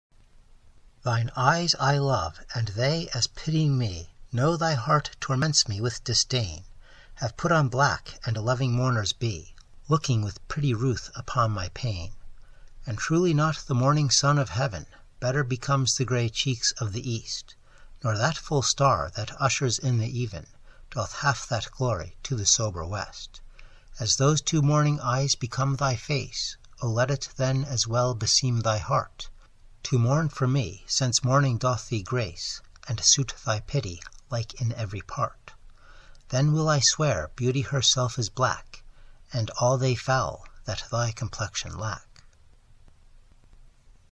"Sonnet 132" by William Shakespeare
Thine eyes I love, and they, as pitying me,
Knowing thy heart torments me with disdain,
Have put on black and loving mourners be,
Looking with pretty ruth upon my pain.
And truly not the morning sun of heaven
Better becomes the grey cheeks of the east,
Nor that full star that ushers in the even
Doth half that glory to the sober west,
As those two mourning eyes become thy face:
O, let it then as well beseem thy heart
To mourn for me, since mourning doth thee grace,
And suit thy pity like in every part.
    Then will I swear beauty herself is black,
    And all they foul that thy complexion lack.